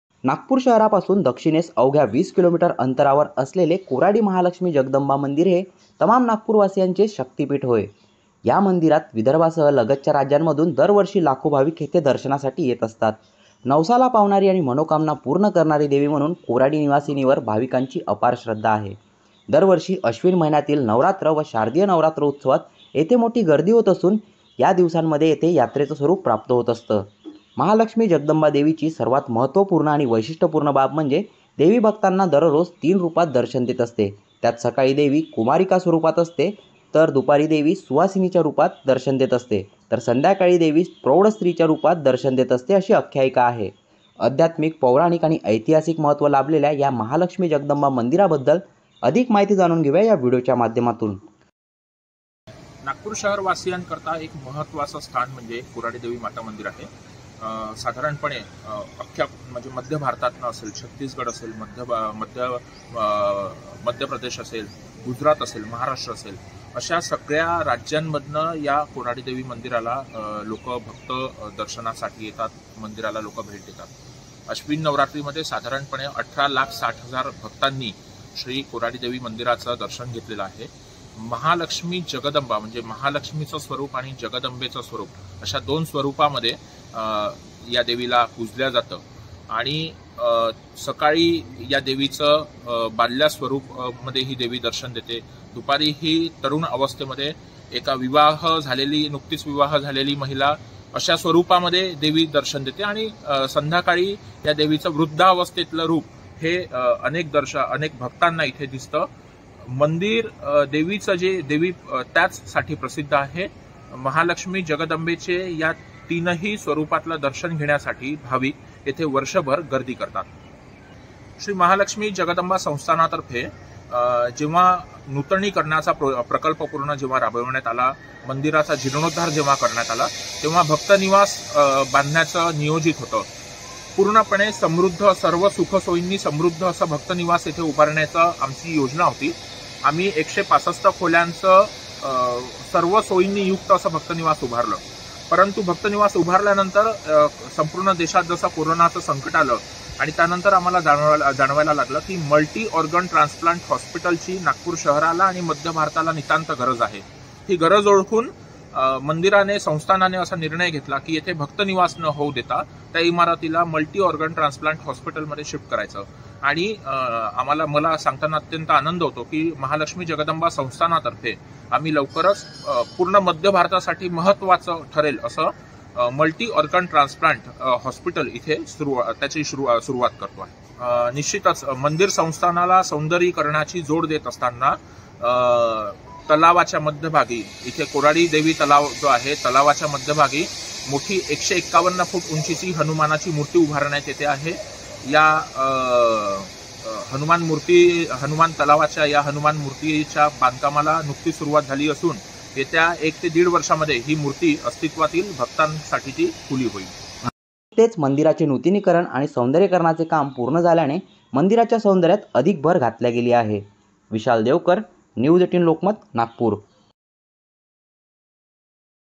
Koradi Mandir| 'या' मंदिरात देवी दिवसातून 3 रूपांत देते दर्शन, घरबसल्या घ्या दर्शनाचा लाभ
नागपूर शहरापासून दक्षिणेस अवघ्या 20 किमी अंतरावर असलेले कोराडी महालक्ष्मी जगदंबा मंदिर हे तमाम भाविकांचे शक्तीपीठ आहे. विदर्भासह लगतच्या राज्यामधून दरवर्षी लाखो भाविक येथे दर्शनासाठी येत असतात. नवसाला पावणारी आणि सर्वांच्या मनोकामना पूर्ण करणारी देवी म्हणून कोराडी निवासिनीवर भाविकांची अपार श्रद्धा आहे. महालक्ष्मी जगदंबा देवी दिवसांतून तीन रुपात भाविकांना दर्शन देते अशी आख्यायिका आहे. घेऊया महालक्ष्मीचं दर्शन.